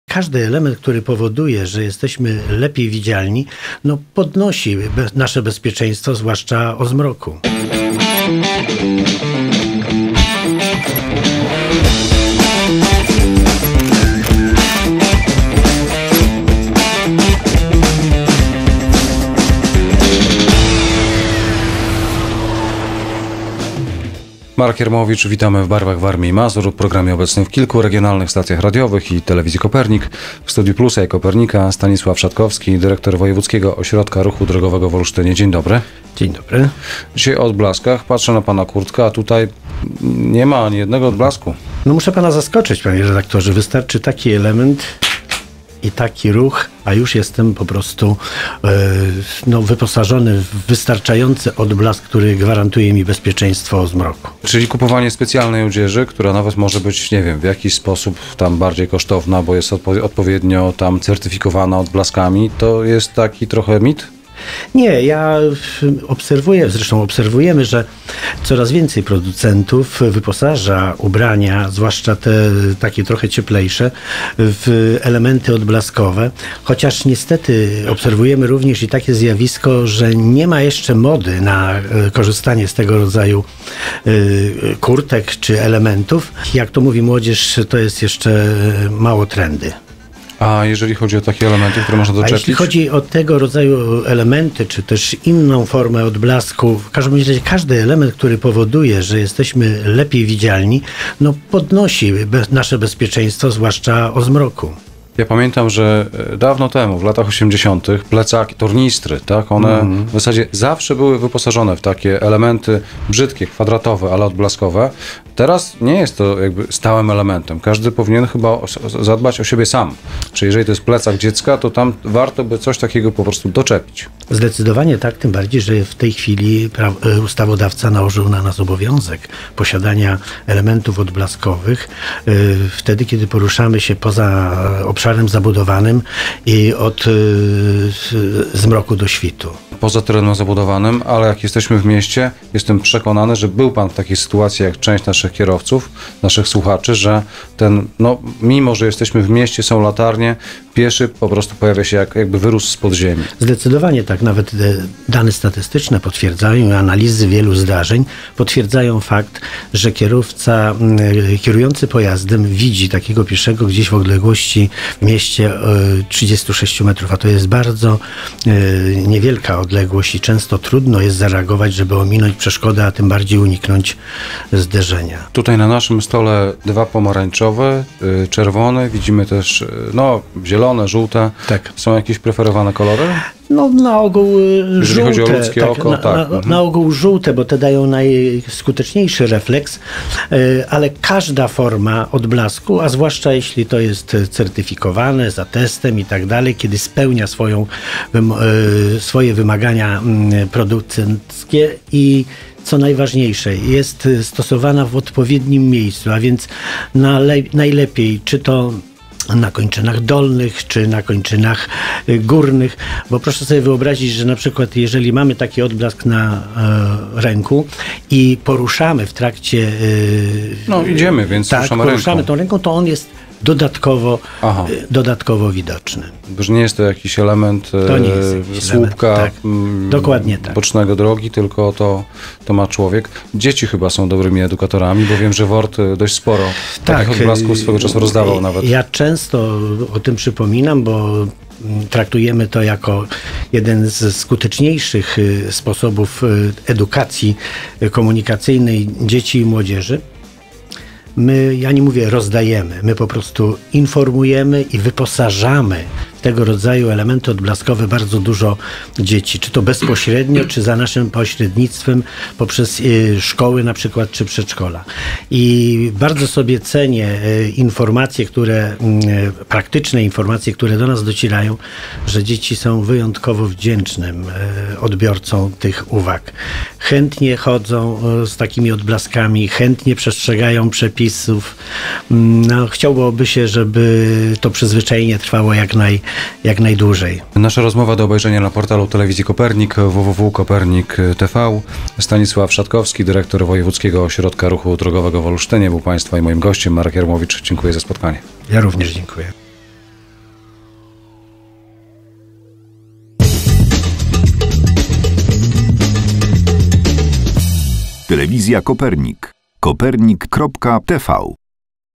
MODA NA BLASK
Czy jest moda na odblaski? Raczej nie, bo wielu pieszych, nawet tych poruszających się po zmroku poza miastem, elementów odblaskowych nie używa. Niestety, bo jak mówią statystyki, wypadków z udziałem niewidzialnych pieszych jest dużo.